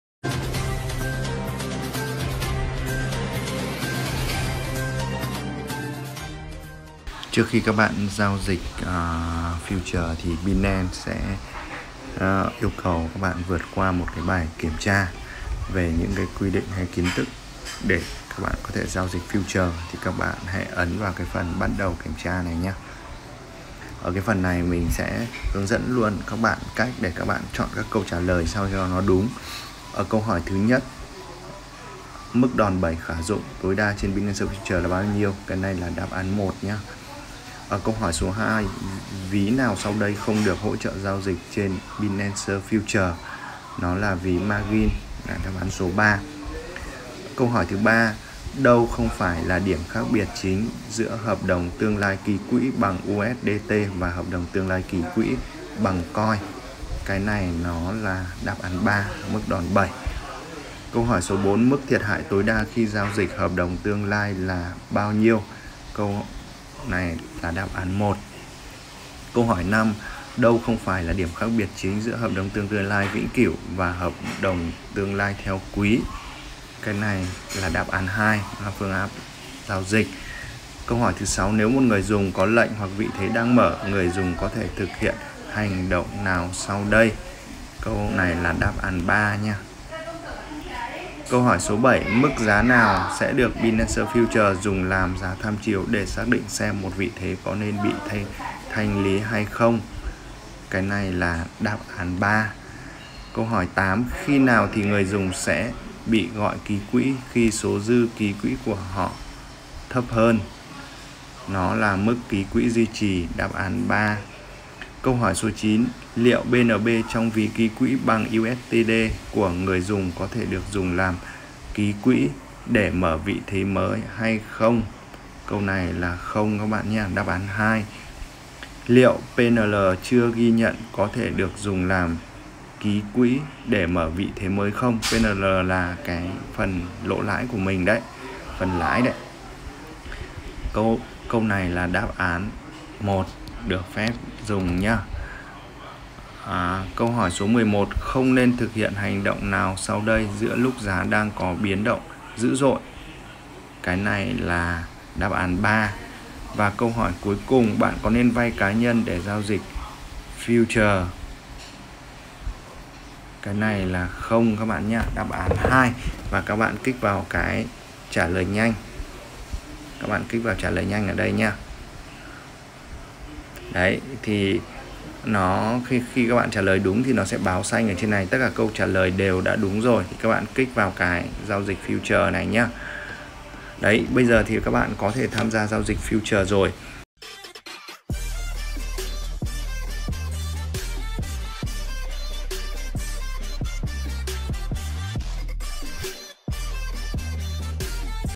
An Brothers Mẹo Vặt | #84 | Cách Vượt Qua Bài Kiểm Tra Để Giao Dịch Phái Sinh (Future) Trên Binance
Cảm ơn các bạn đã ủng hộ kênh của mình. Mình mong rằng những chia sẻ của mình sẽ giúp ích được cho các bạn.